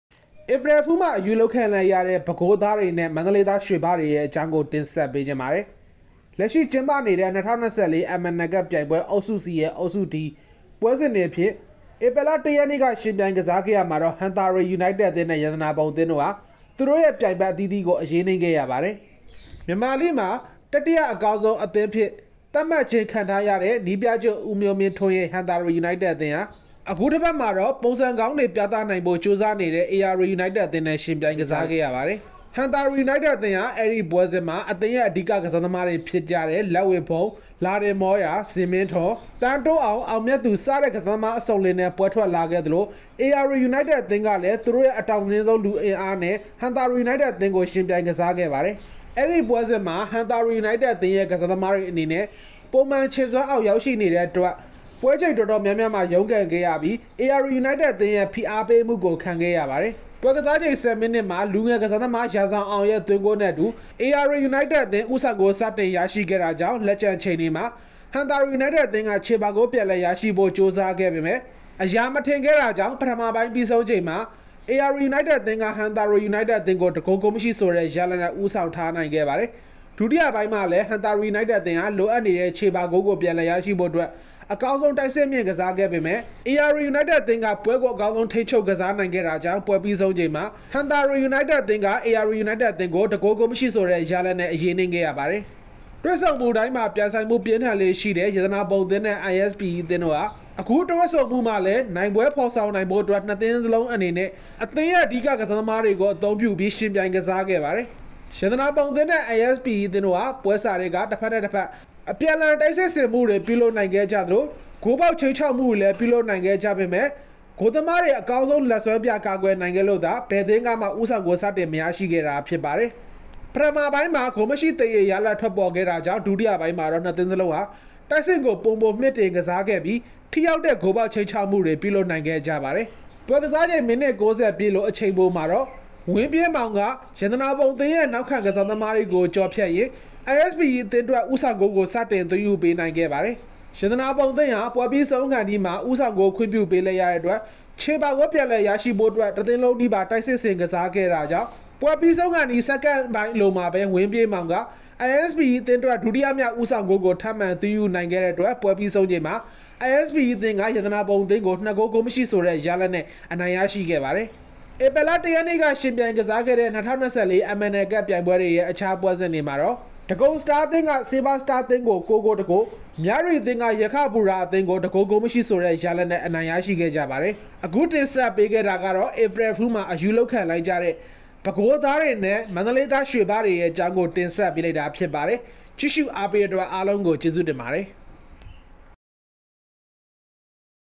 ဧပရယ်ဖူးမှာ အရူးလုပ်ခံရတဲ့ ပဲခူးသားနဲ့ ရွှေဘများ။
ဧပြီလ ၁ရက်က ယှဉ်ပြိုင်ကစားခဲ့တဲ့ ၂၀၂၄ MNL CUP ပြိုင်ပွဲမှာတော့ ဟံသာဝတီယူနိုက်တက်အသင်းက ဧရာဝတီယူနိုက်တက်အသင်းကို အရေးနိမ့်ခဲ့ရသလို ရတနာပုံအသင်းကလည်း ISPE အသင်းကို ရှုံးနိမ့်ခဲ့ရပါတယ်။